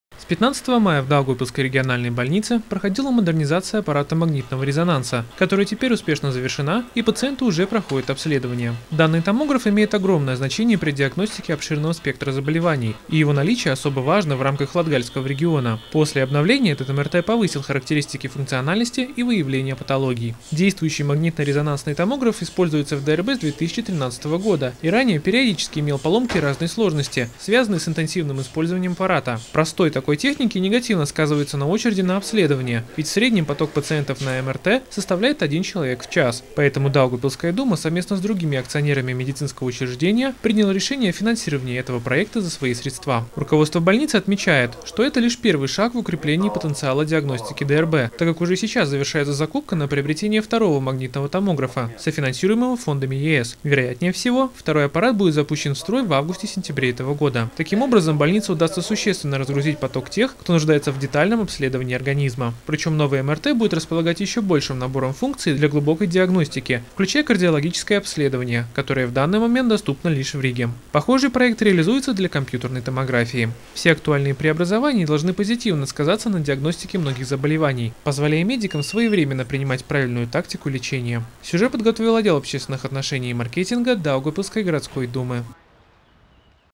В ДРБ успешно модернизирован магнитно-резонансный томограф
С 15 мая в Даугавпилсской региональной больнице проходила модернизация аппарата магнитного резонанса (МРТ), которая теперь успешно завершена и пациенты уже проходят обследования. Данный томограф имеет огромное значение при диагностике обширного спектра заболеваний и его наличие особо важно в рамках латгальского региона. После обновления этот МРТ повысил характеристики функциональности и выявления патологий.